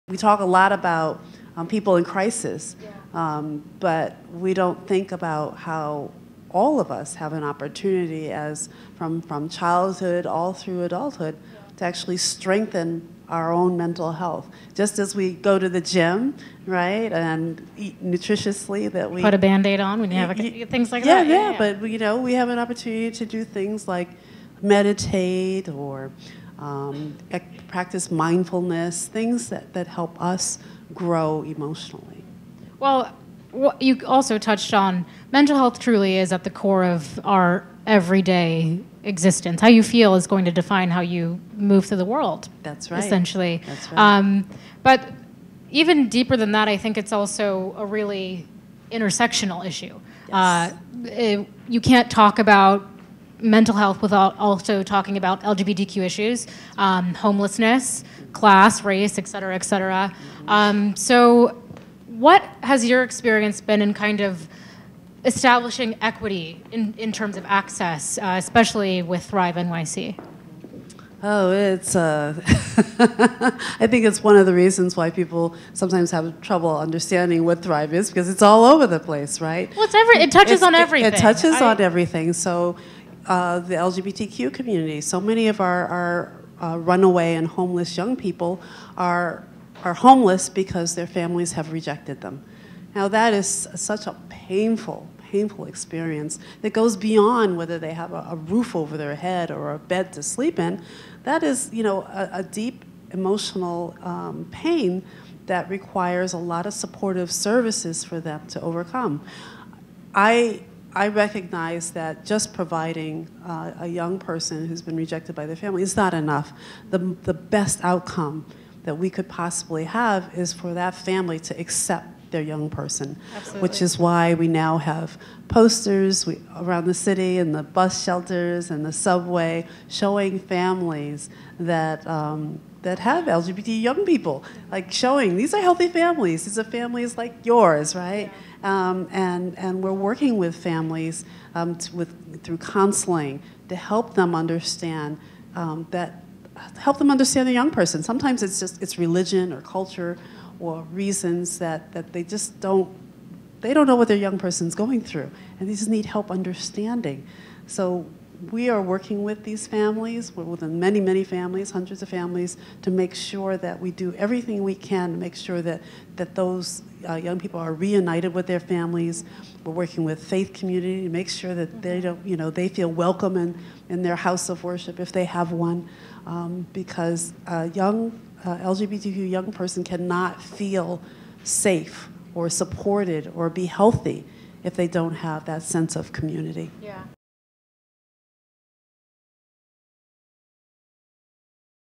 NYC First Lady Chirlane McCray on How to Tackle the Stigma Around Mental Illness
Chirlane McCray explained why she has made mental health issues her mission since her husband, Bill DeBlasio, was elected mayor of New York City in 2013: “Every single one of us is touched by (mental health) in some way.”

Speaking Tuesday at TheWrap’s Power Woman Breakfast New York City, McCray told them. Executive Editor Whembley Sewell that she hope her ThriveNYC mental health program will empower people to work on their mental health “just as we go to the gym” to work on physical health.